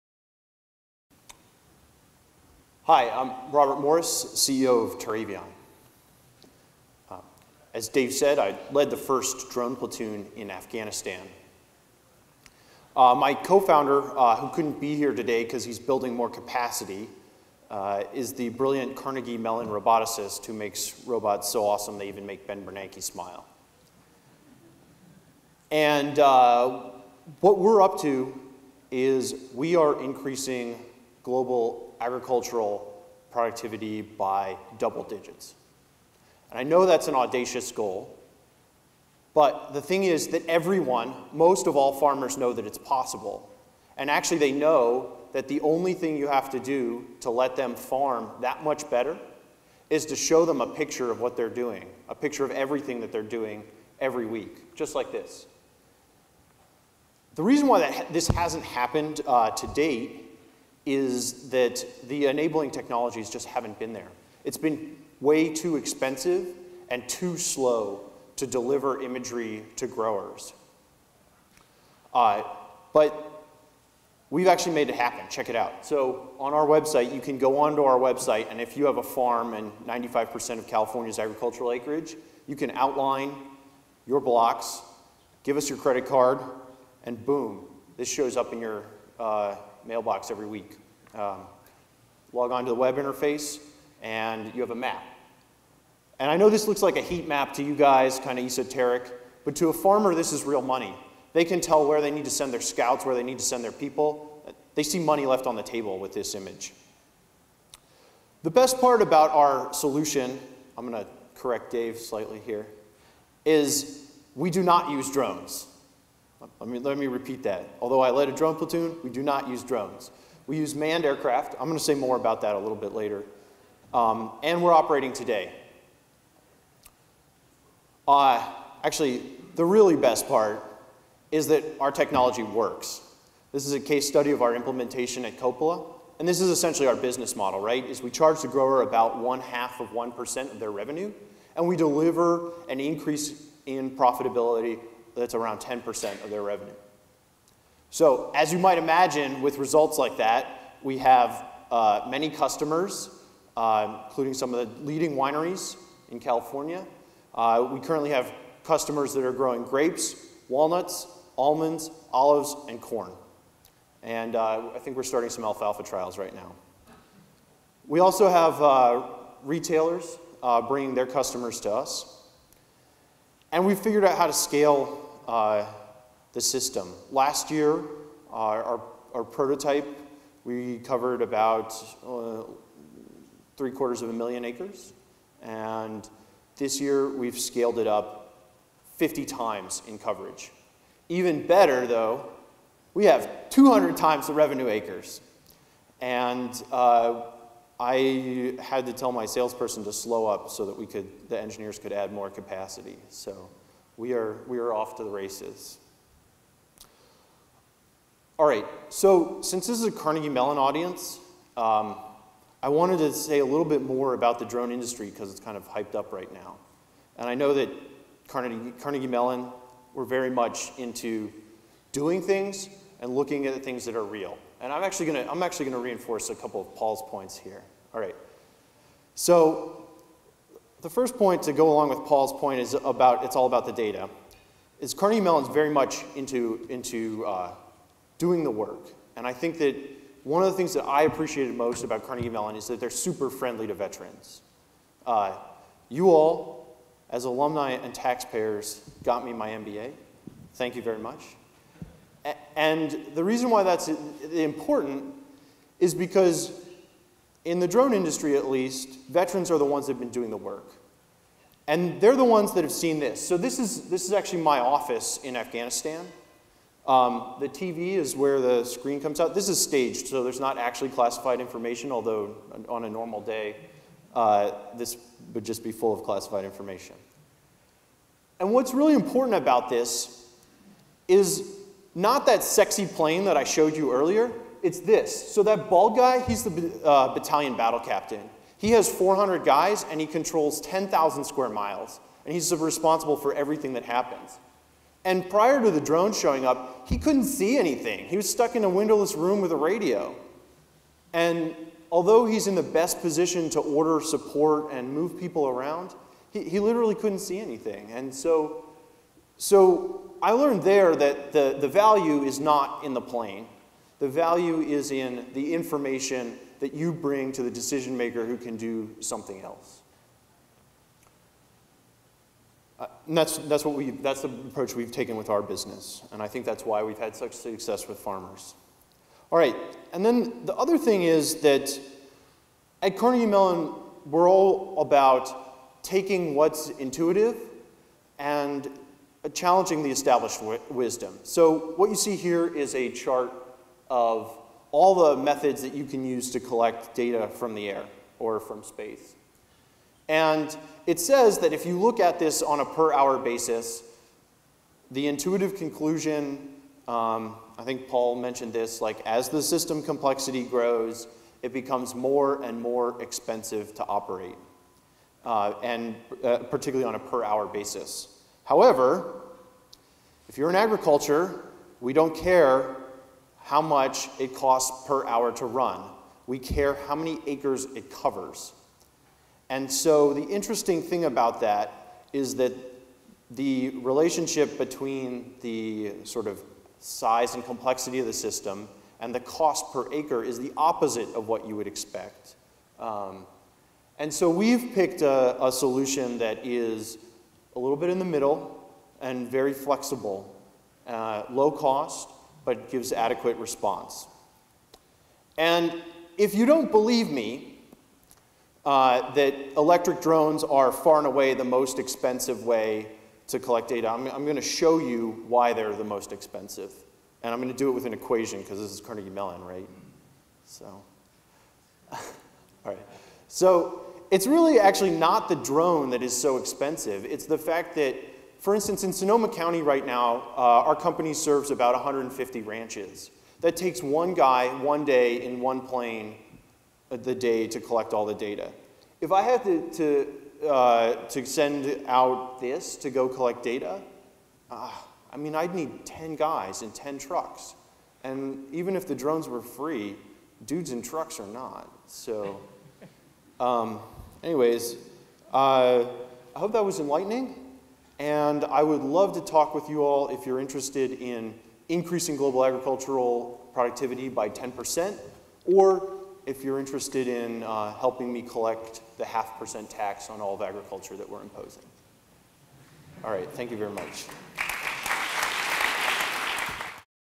TerrAvion
Why the Agricultural 'Drone' Market is Winner Takes All and Why the Winner Will Come from CMU

TerrAvion makes real-time web maps for agriculture. Growers use these maps to increase farm productivity by over 10%.